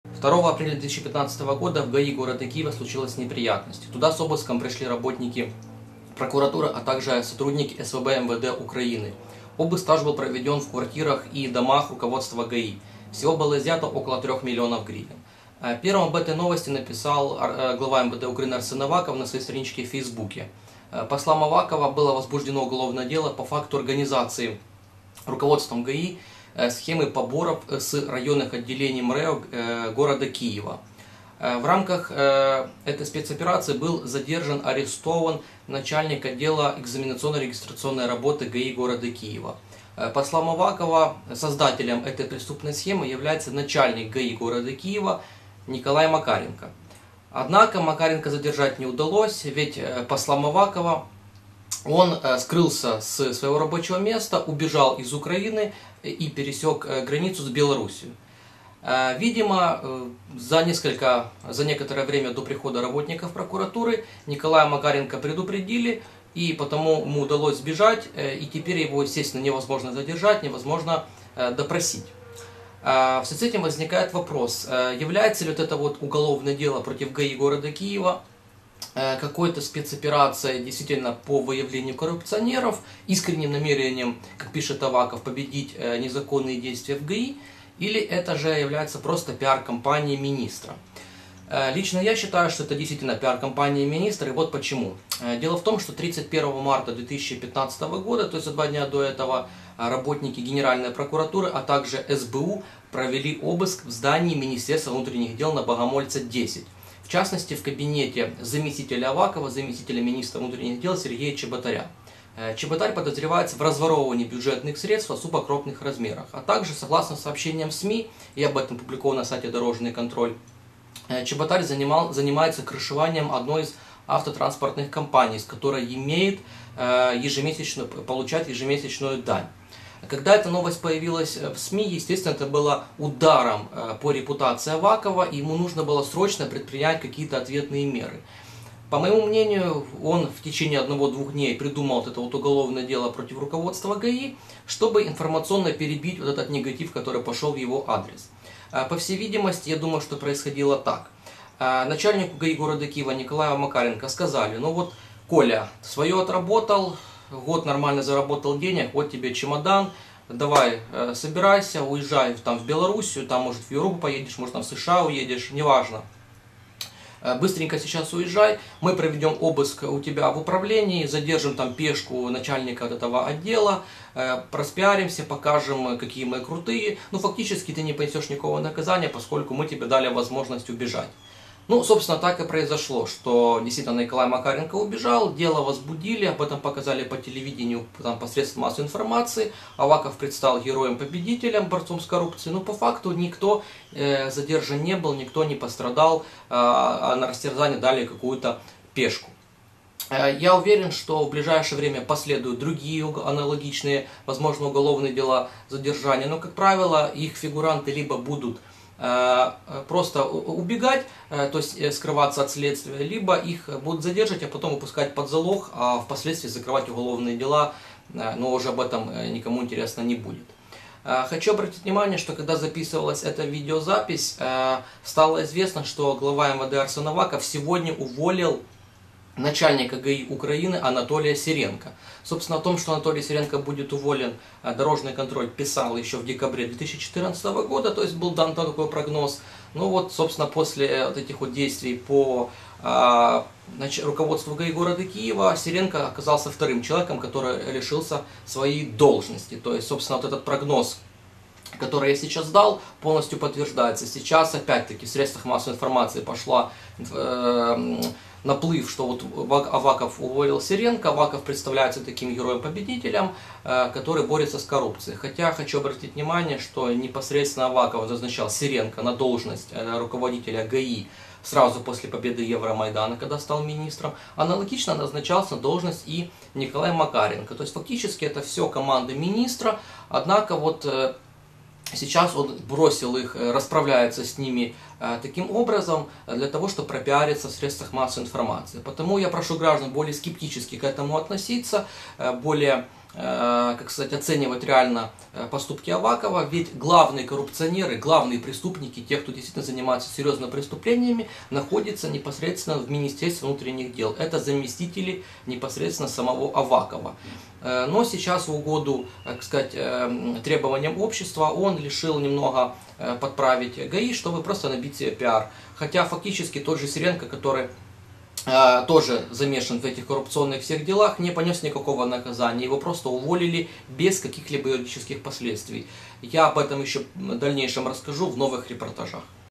Пиар Авакова на увольнении руководства ГАИ
Последние несколько дней, украинские СМИ переполнены новостями о борьбе главы МВД Арсена Авакова с коррупцией в Госавтоинспекции. 2 апреля 2015-го работники СВБ МВД и прокуратуры провели обыск в здании УГАИ Киева - был задержан начальник отдела регистрационно-экзаменационной работы, а руководитель УГАИ Николай Макаренко обьявлен в розыск. Сегодня, 4 апреля, уволен начальник Департамента ГАИ Анатолий Сиренко...